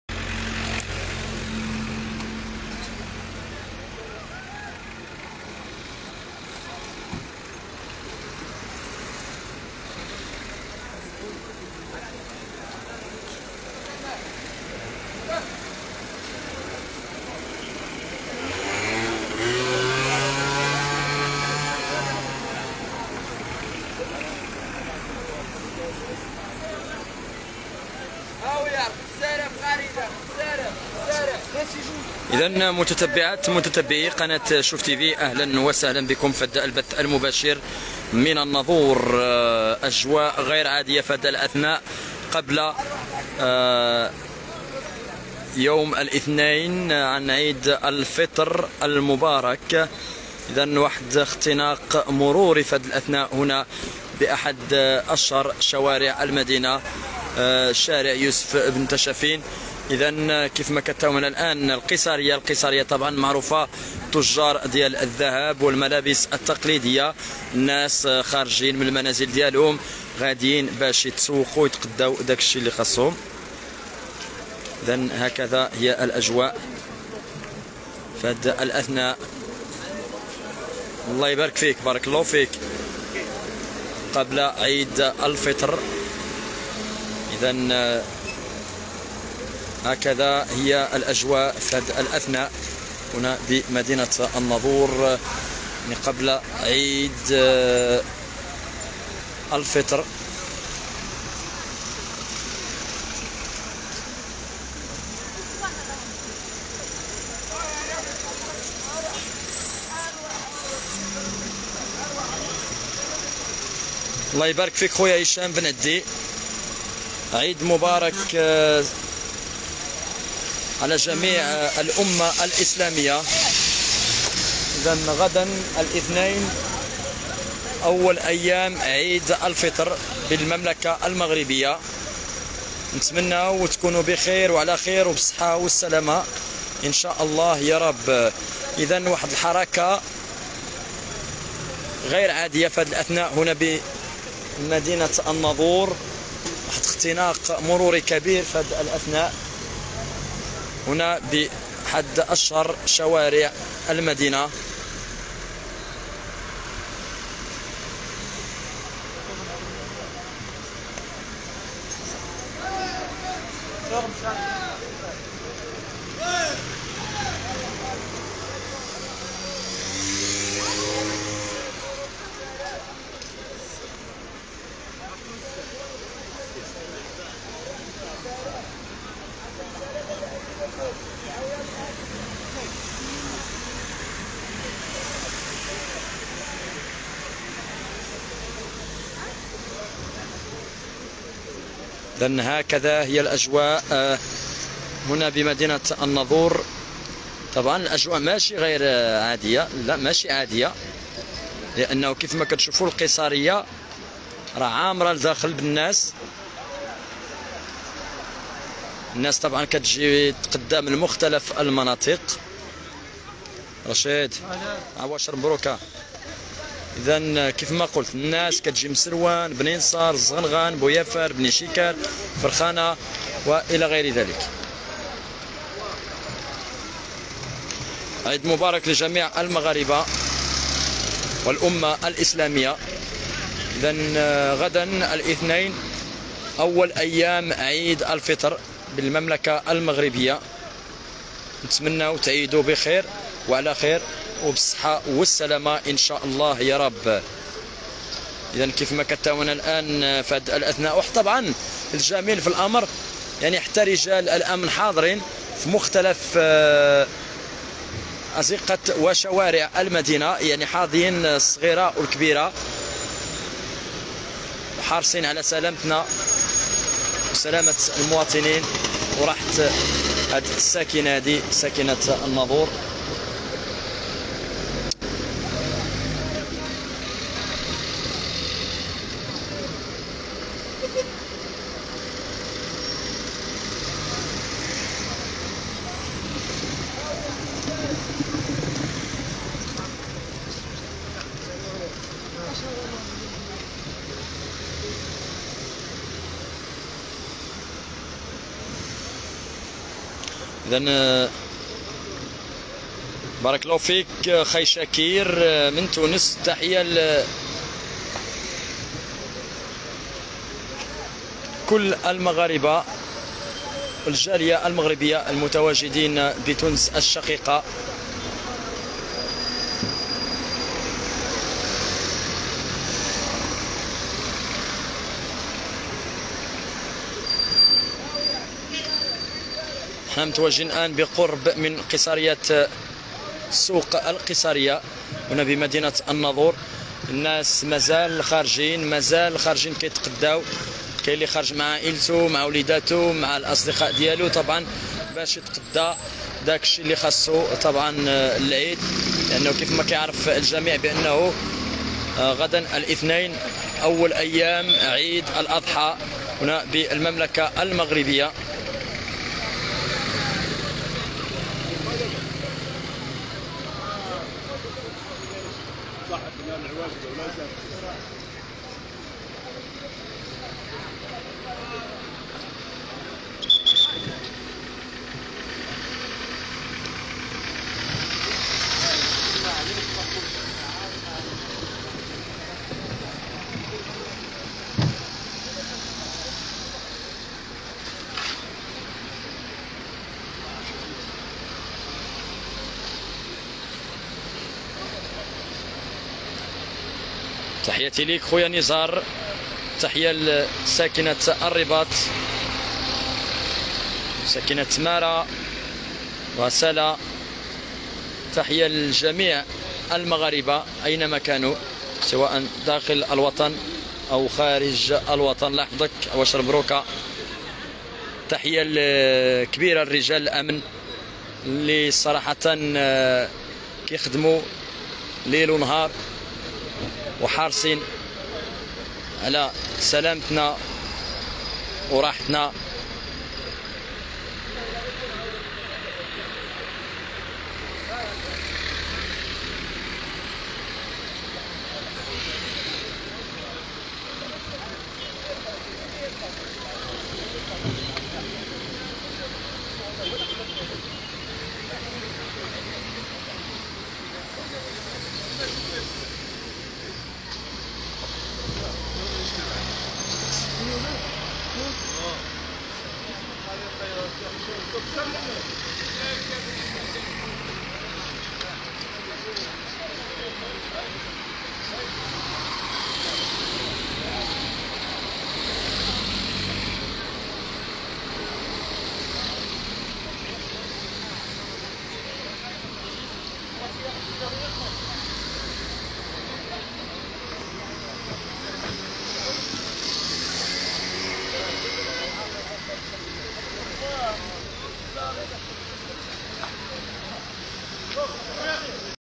من الناظور:اختناق مروري كبير بعد منتصف الليل ورواج تجاري بعد إعلان يوم الإثنين عيد الفطر بالمغرب
مباشرة من الناظور:اختناق مروري كبير بعد منتصف الليل ورواج تجاري بعد إعلان يوم الإثنين عيد الفطر بالمغرب